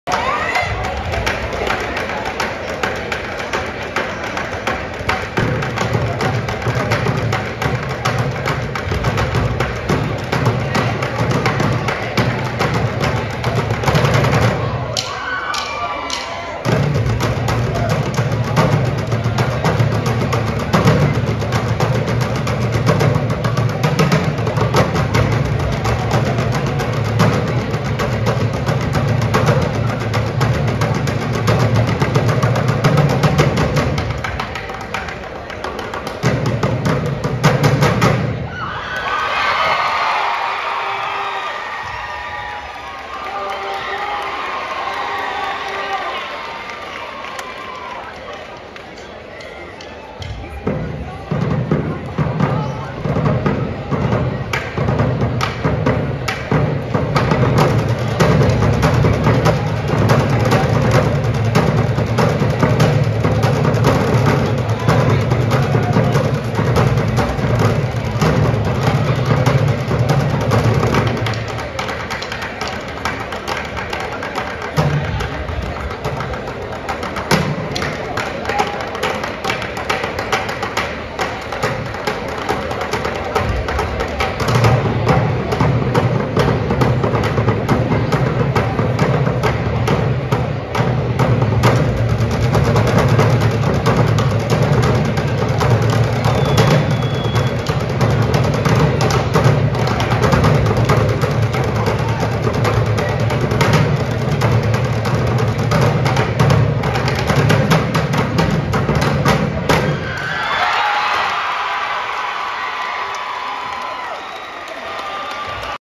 Nederland High Schools Drum line
Playing buckets at pep rally. Oct 21, 2022